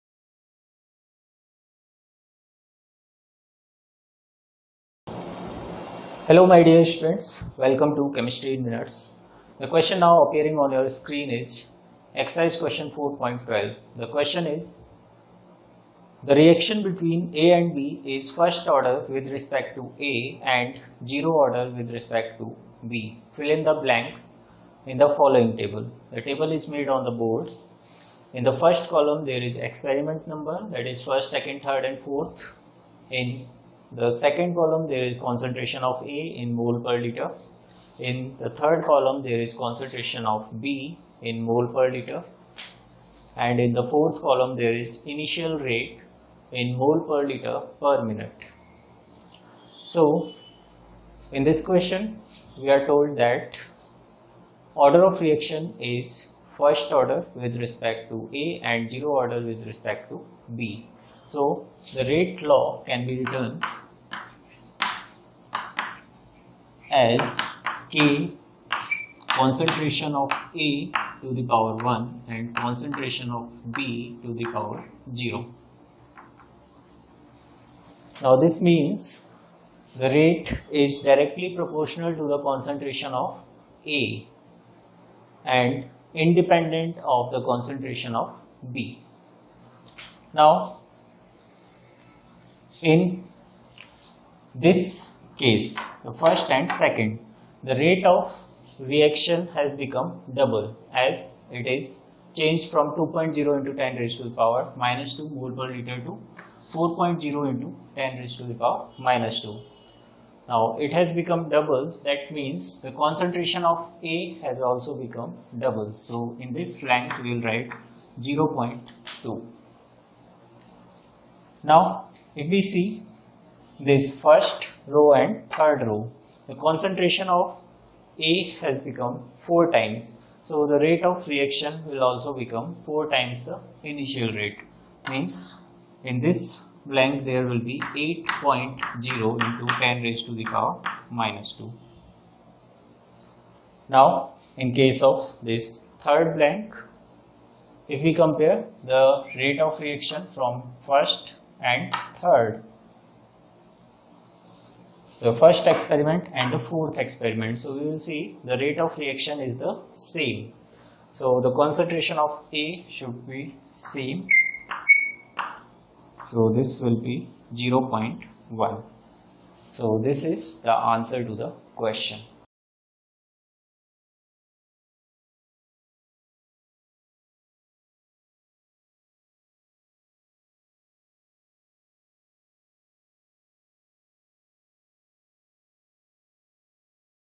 CHEMICAL KINETICS | Exercise Questions 3.12 | CLASS 12 CHEMISTRY | NCERT CHAPTER 3
Solution to NCERT Exercise Question chemistry class 12 CHEMICAL KINETICS Exercise Question 3.12